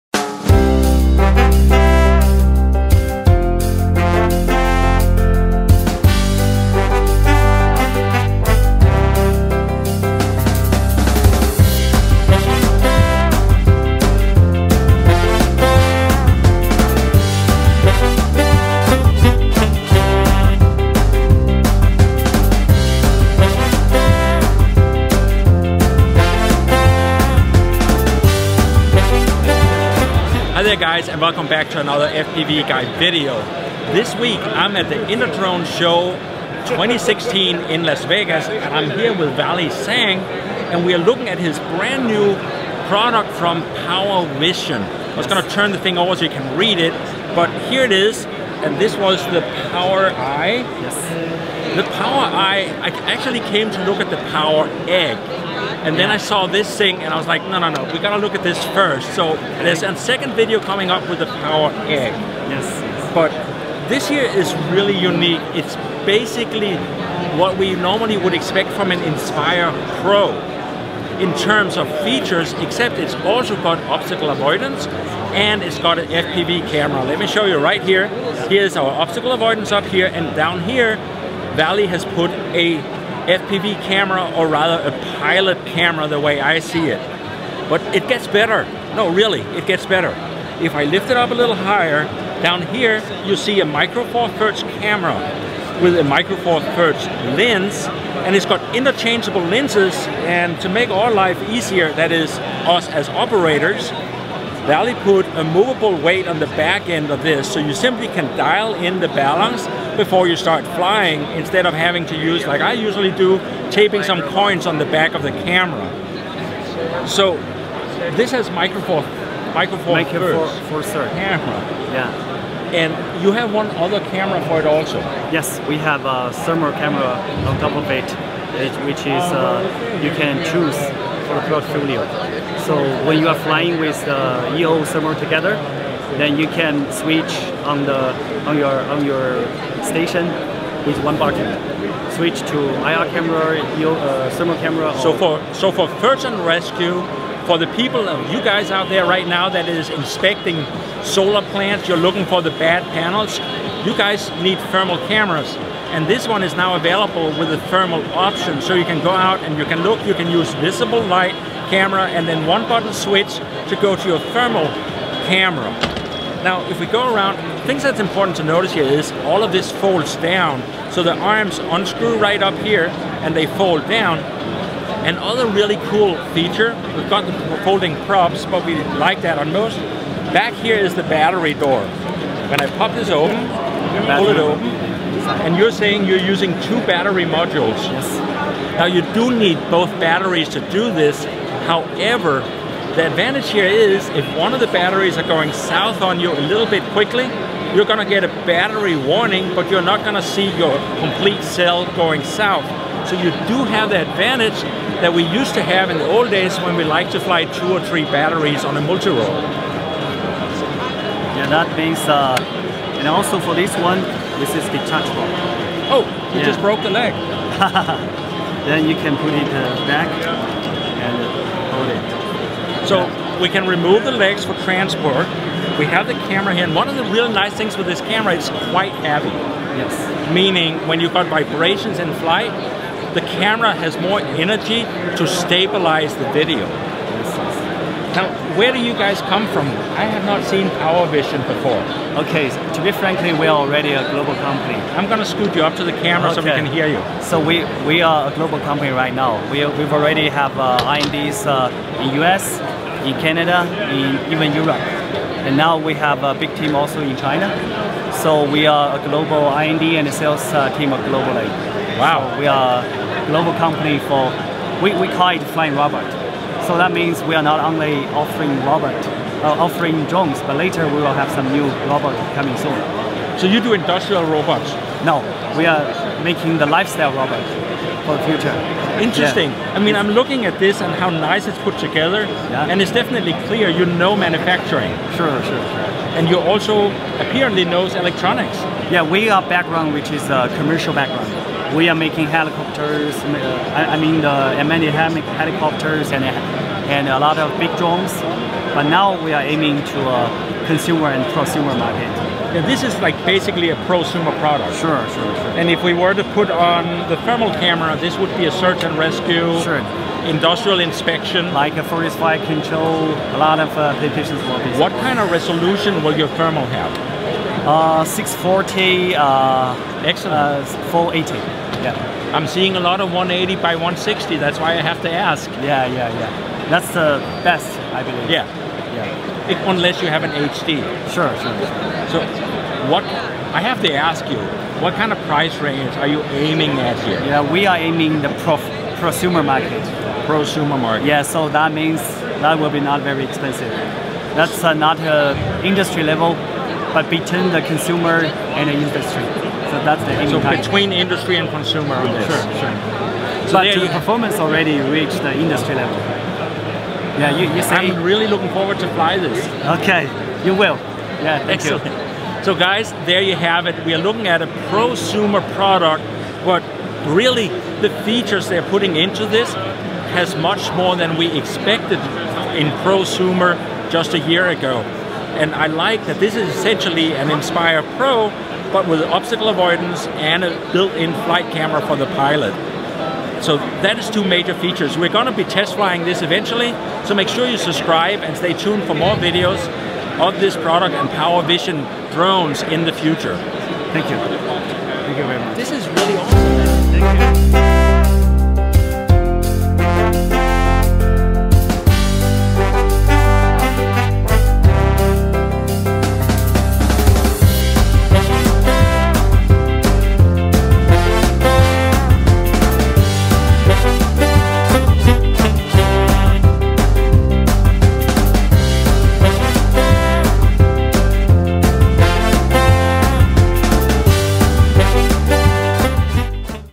InterDrone 2016 - PowerEye m43 camera drone
The new PowerEye m43 camera drone from PowerVision
5000m control range, m43 camera with interchangeable lenses
audio obstacle avoidance / detection and note the dedicated pilot FPV camera.